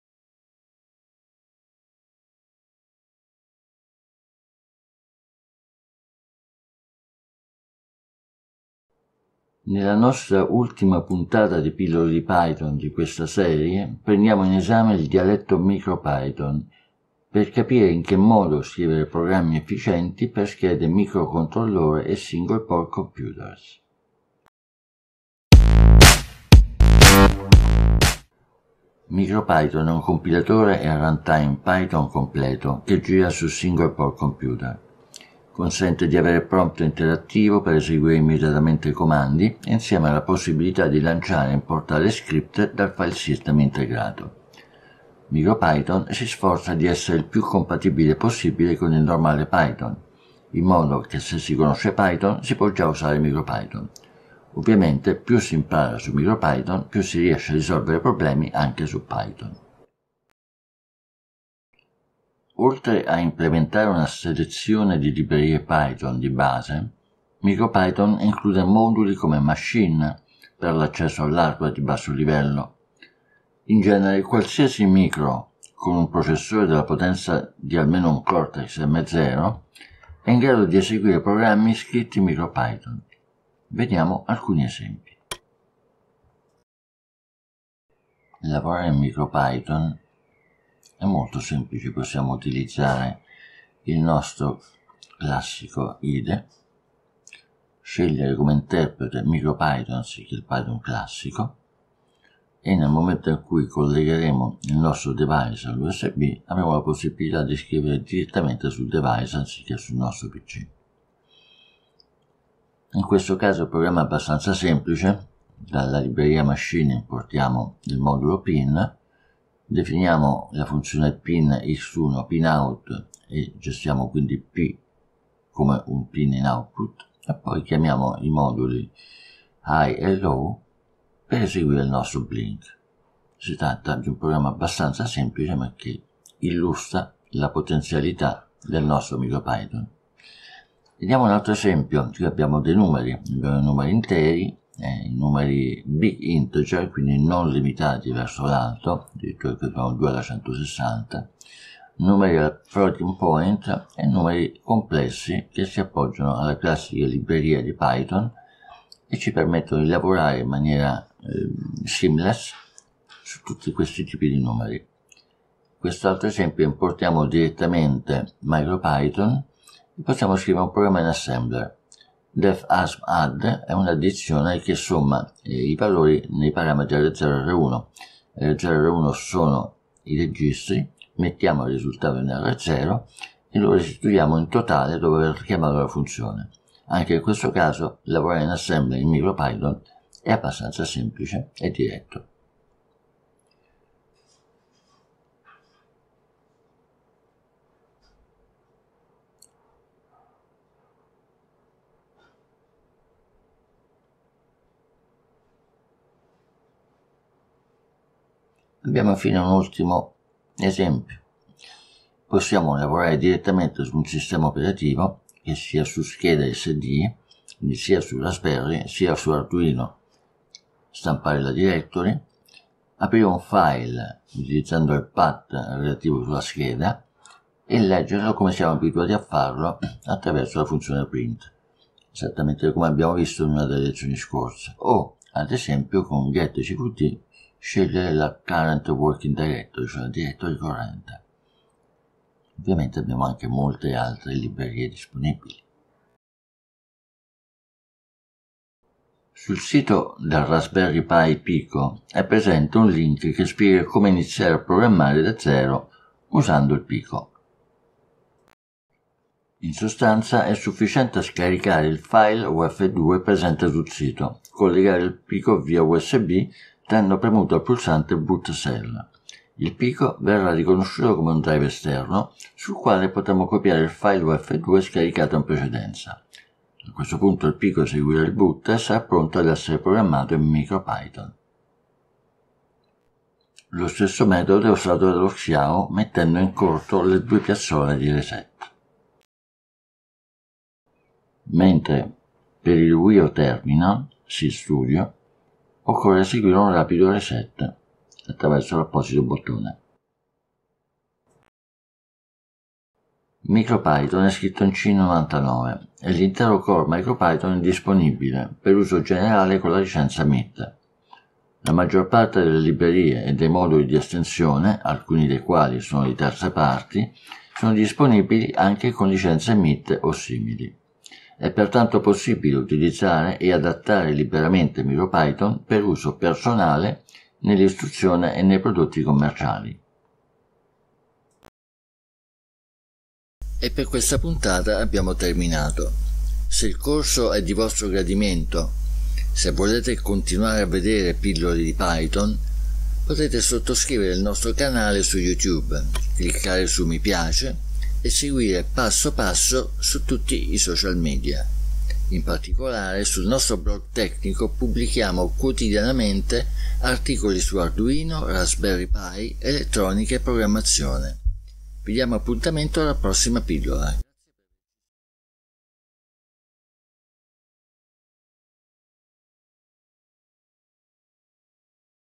Pillole di Python #039 - MicroPython
Micropython e Raspberry PI Pico
Nella nostra ultima puntata di Pillole di Python di questa serie, prendiamo in esame il dialetto Micropython per capire in che modo scrivere programmi efficienti per schede microcontrollore e single board computers.

Raspberry PI 4 kit

Raspberry PI Pico

STM32 Nucleo32

Seeed Studio Wio Terminal
Grove Shield for Xiao
Teensy 4.0

Seguici per non perdere le prossime novità